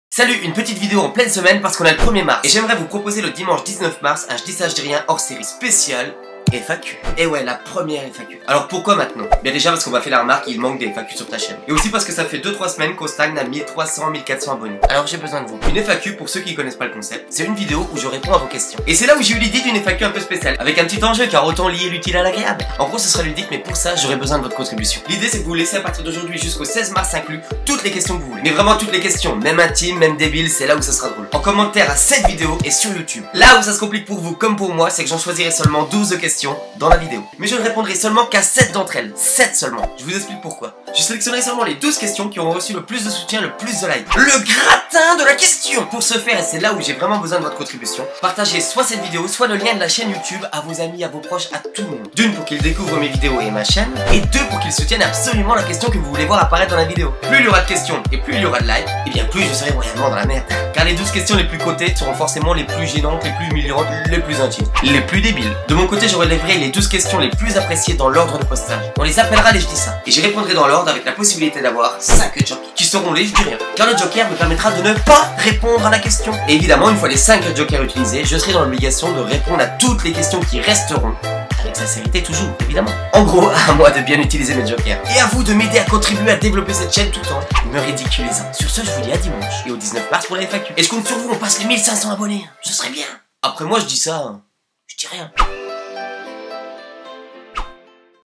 Une Première FAQ ça vous tente?
Salut! Salut!
A l'occasion du "Je dis ça... je dis rien!" du Dimanche 16 MARS, j'ai décidé de vous proposer ma 1ère FAQ! :-)

2 Objectifs:
-Faire connaissance avec vous!
-Et que vous contribuez à faire vivre cette chaîne en essayant d'atteindre les 1500 abonnés pour le 19 MARS!!!

LA REGLE EST SIMPLE!
-Je prendrai les 12 QUESTIONS qui auront le plus de LIKES dans la videos du 19 MARS.
-Je prendrai ensuite ces 12 questions "PREMIUM" dans l'ordre chronologique et j'y répondrai (AVEC SINCERITE ET HUMOUR!!!)
-J'aurai le droit à 5 JOKERS (LES JeDisRien, qui me permettront de ne rien dire, et donc de ne pas répondre à la question.
-Une fois les 5 JOKERS utilisés...je ne pourrai plus me défiler pour les questions restantes!!!

Au final, 12 questions...7 REPONSES...5 JOKERS...et 1 BONNE DOSE D'HUMOUR!!

LÂCHEZ-VOUS, PARLEZ DE LA CHAÎNE et INVITEZ LES GENS A LIKER LES QUESTIONS QUI VOUS TIENNENT A COEUR!!

Des bisous les photos

Ecriture/ Réalisation/ Montage : ROMAIN HENRY

THEME: Préparation de la première FAQ. Qui sera le JeDisCaJeDisRien du 16 MARS!